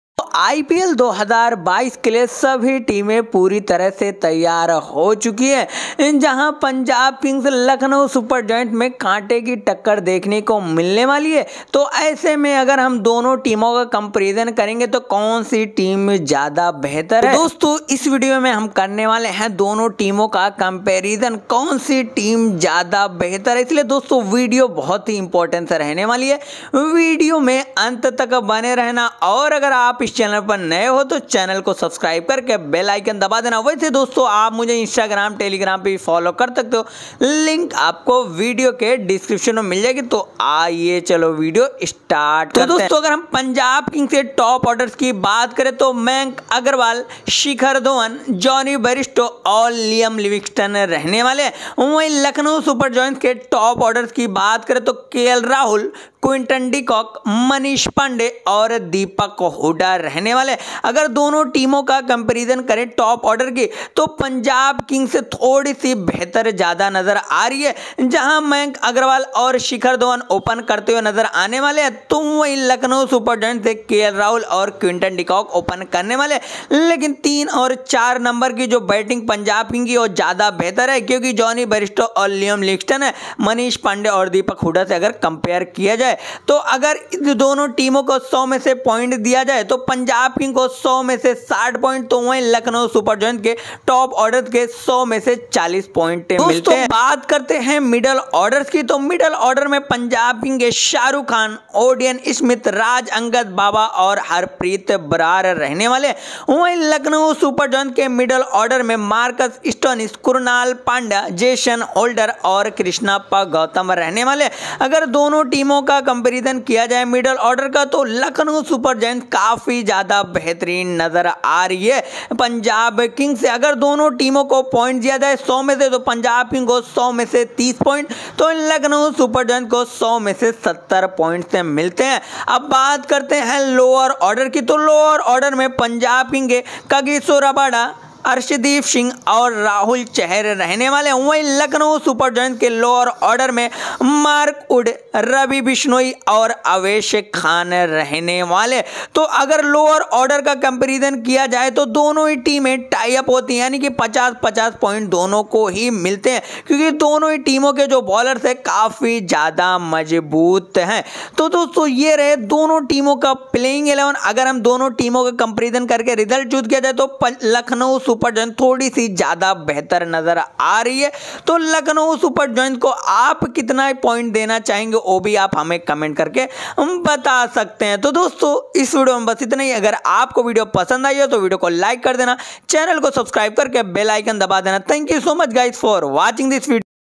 IPL 2022 Teams Comparison - PBKS vs LSG Team Comparison || PBKS vs LSG playing XI
IPL 2022 Teams Comparison
Lucknow Super Giants vs Punjab Kings Comparison
LSG vs PBKS playing 11